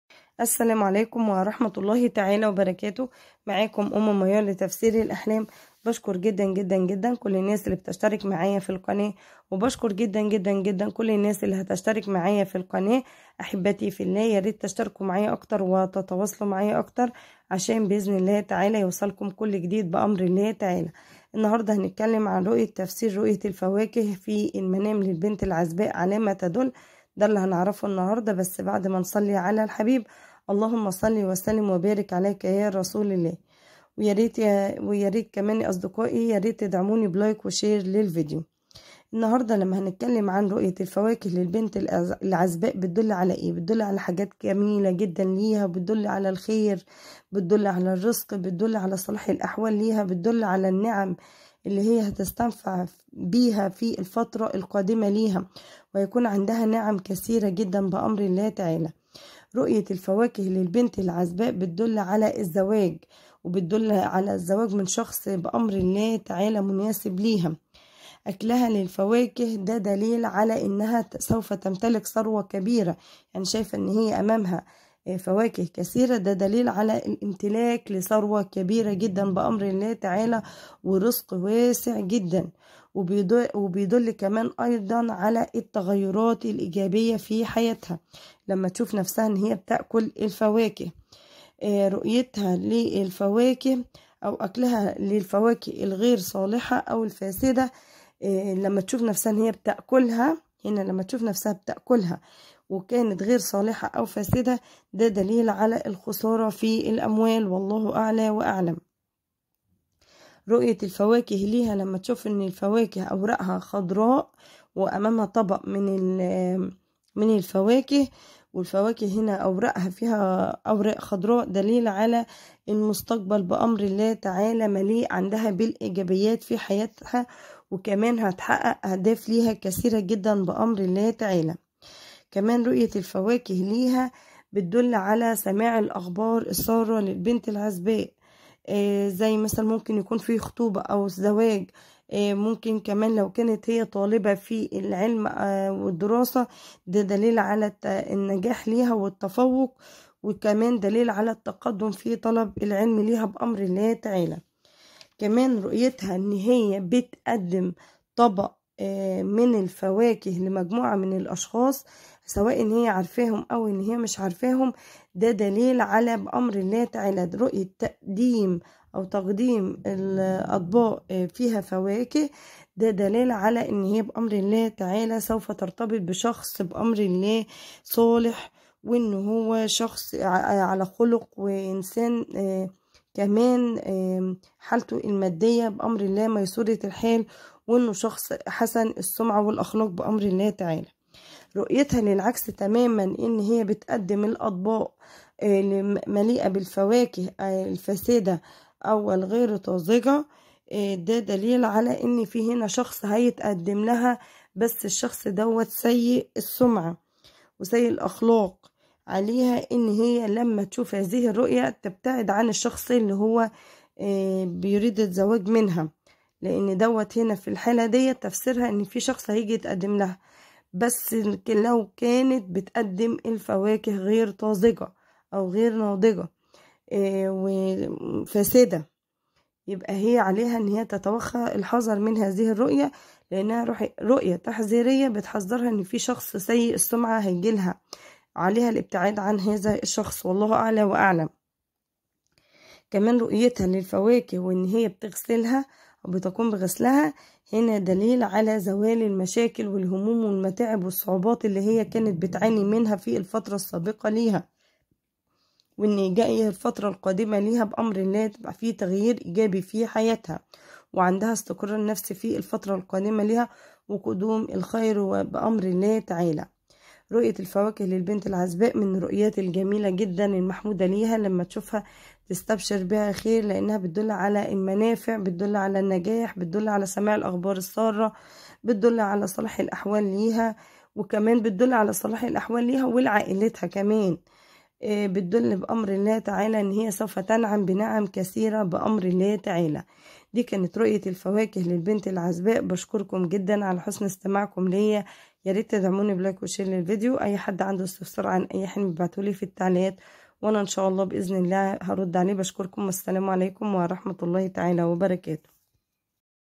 تفسير حلم رؤية اكل الفواكه للبنت العزباء في المنام 😍
يدل علي الخير والرزق وسماع الاخبار الساره